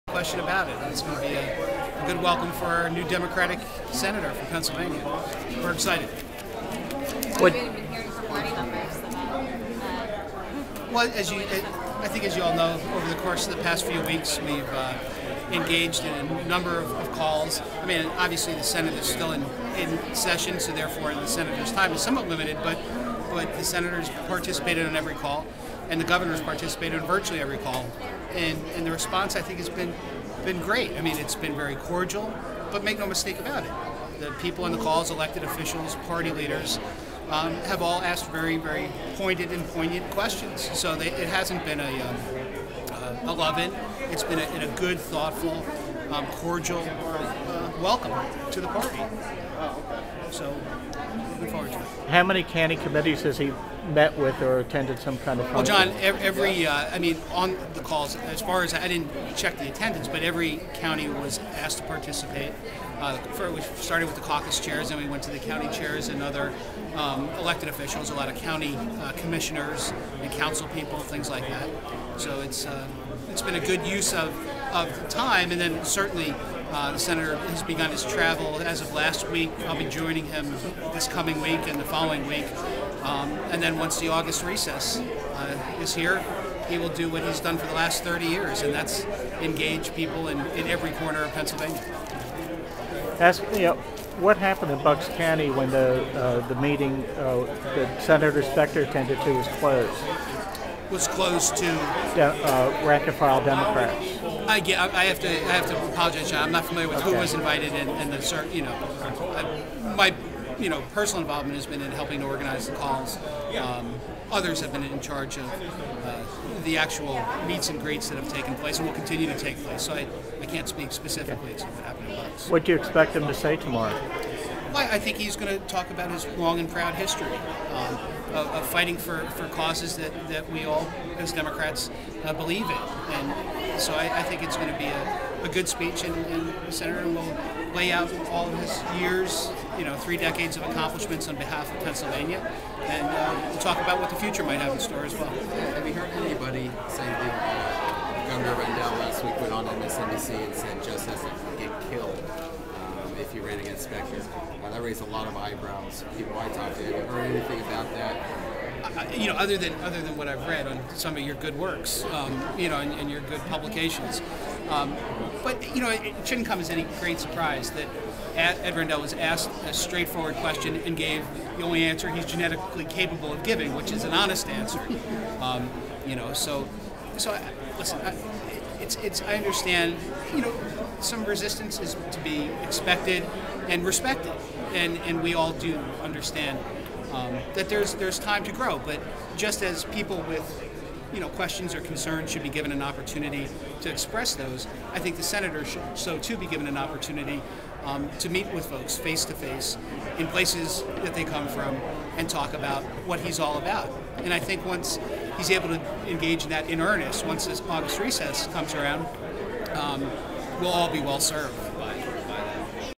TJ Rooney
TJ speaks about the 2010 Pennsylvania Senate race with media at the state Party meeting in Pittsburgh.